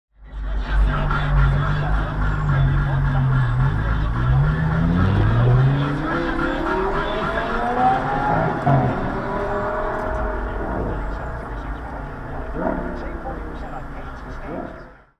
[NB Macleod Trail @ 5 Ave] Pedestrians? What pedestrians?
L8R BRA doesn't have time to wait for you schmucks on foot. He took off revving his engine only to stop at the next set of lights.